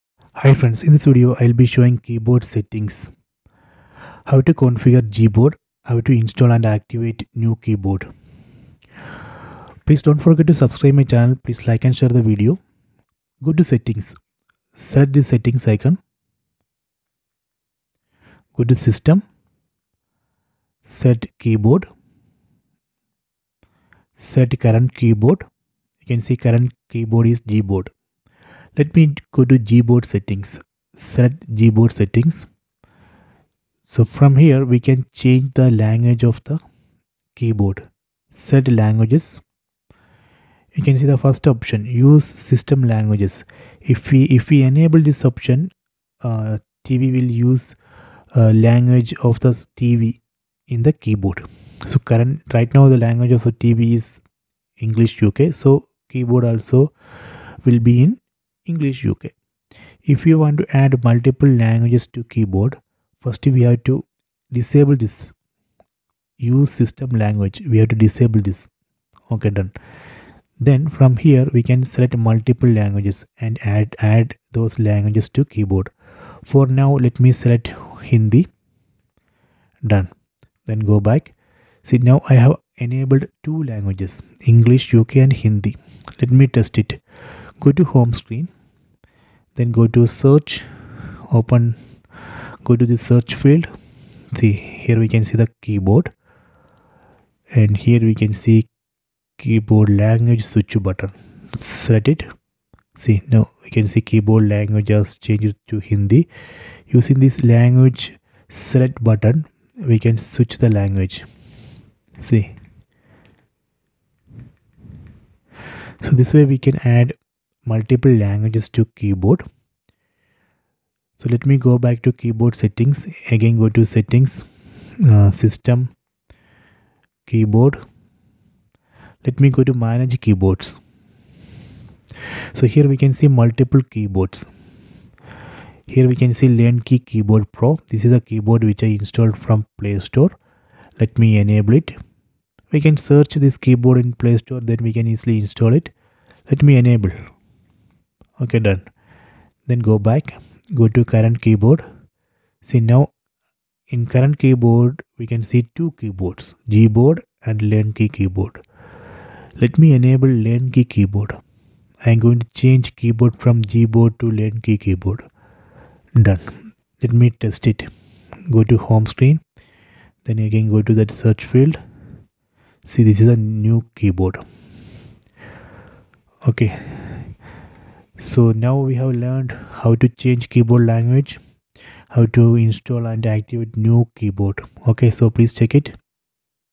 OnePlus Google TV : Keyboard Settings | Gboard Settings in Google TV | Add new Keyboard
change keyboard language, add new keyboard, add multiple keyboard languages, install new keyboard, change Gboard keyboard, Gboard keyboard settings,